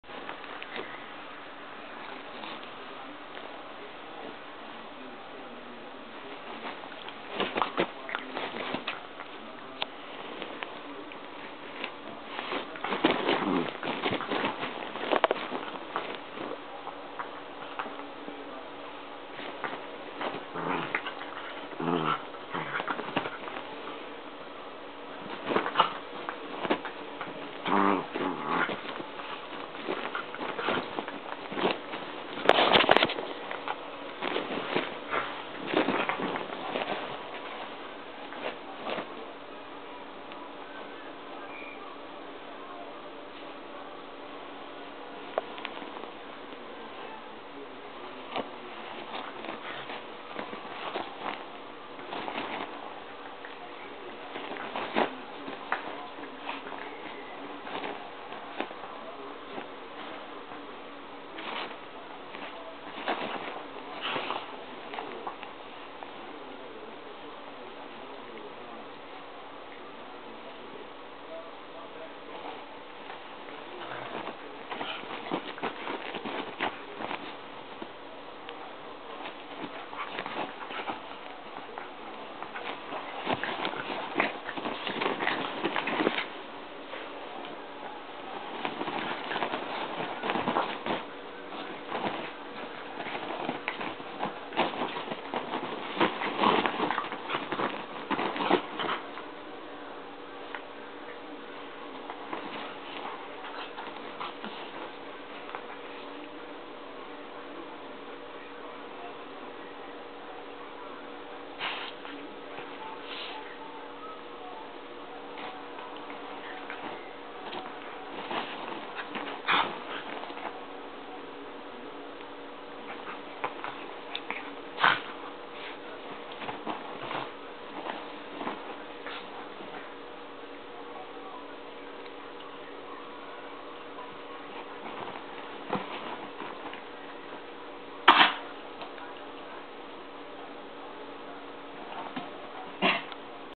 Min Pin wrestling
the small one is vicious usually.